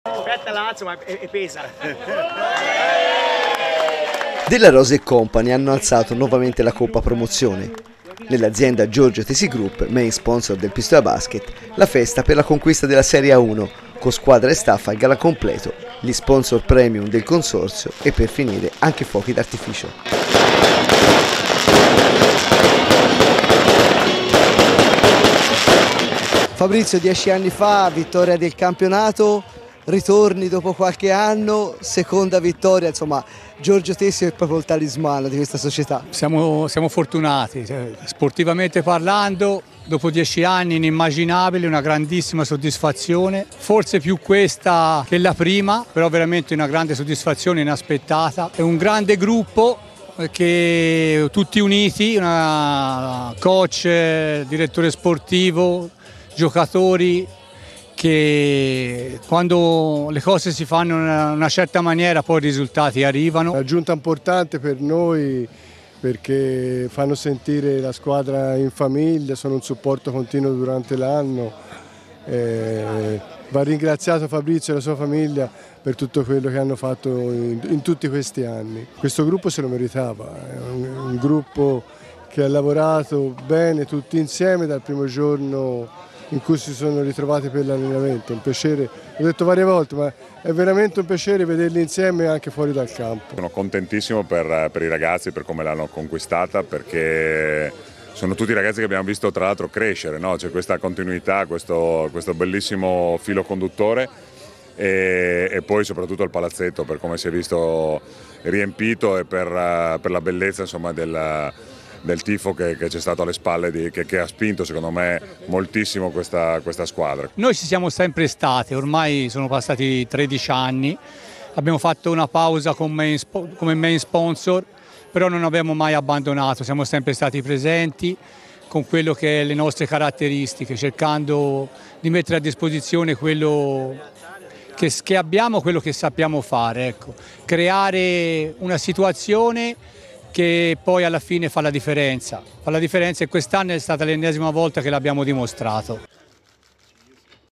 GTG, festa promozione dal main sponsor
Della Rosa e company hanno alzato nuovamente la coppa promozione.Nell'azienda Giorgio Tesi Group, main sponsor del Pistoia Basket, la festa per la conquista delle serie A1 con squadra e staff al gran completo, gli sponsor premium del consorzio e per finire anche i fuochi d'artificio...